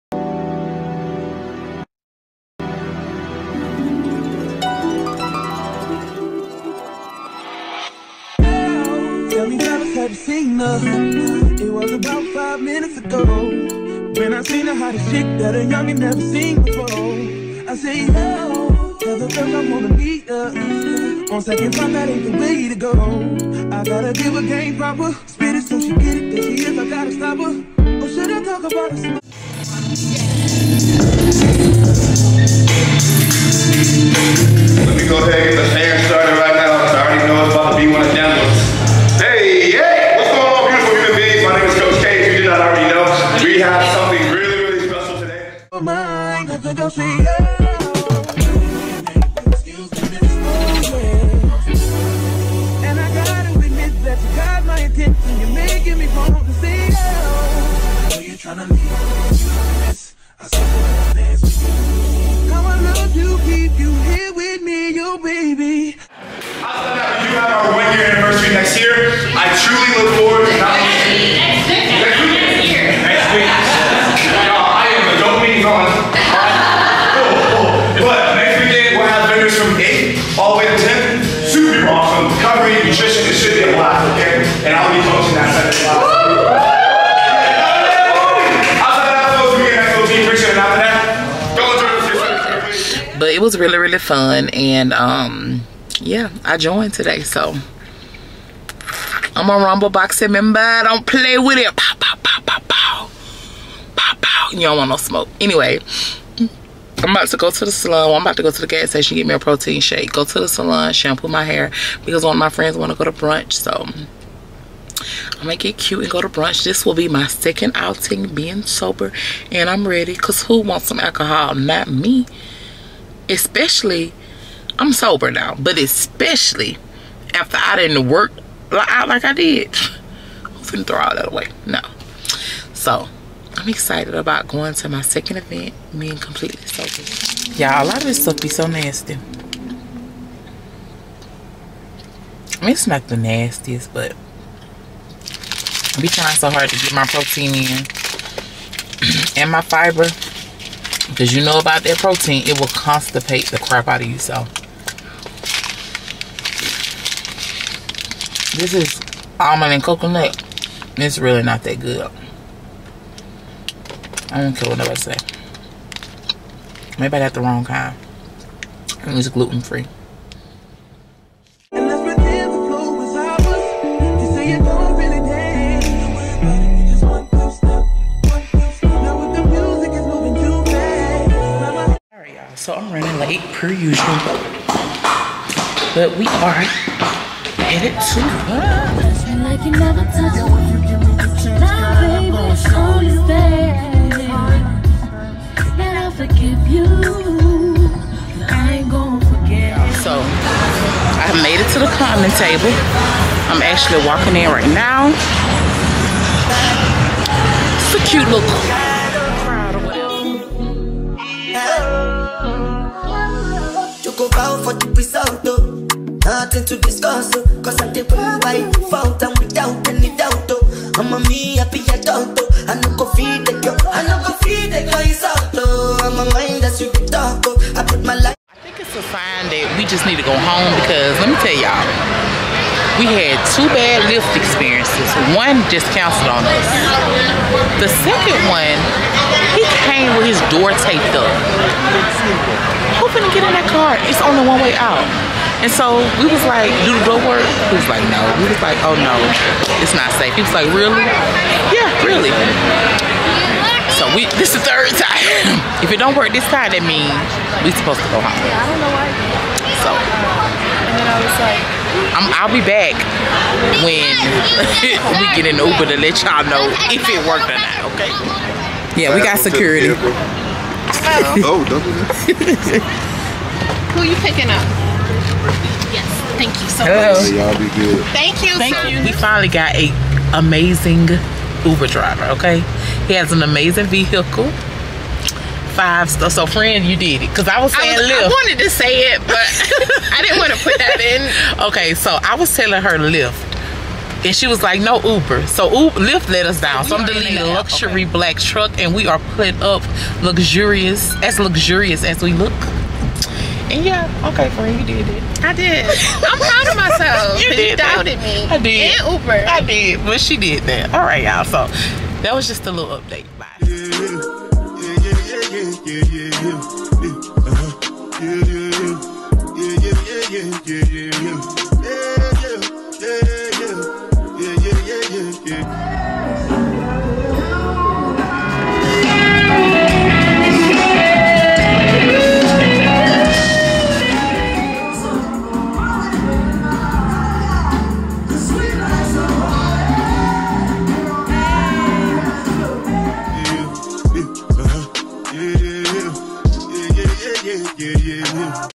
Day In My Life| I Joined A Boxing Class+Sunday Funday in Dallas
Spend the day with me! I joined Rumble boxing and it was so fun, the session was extremely intense. The first 30 mins was strength training and the last 30 minutes was boxing!  We went to brunch at the Common Table which is always a good time! This was my second outing being 100% sober and I am so proud of myself! Later we went to neo soul Sundays at Lava Catina and enjoyed the live band.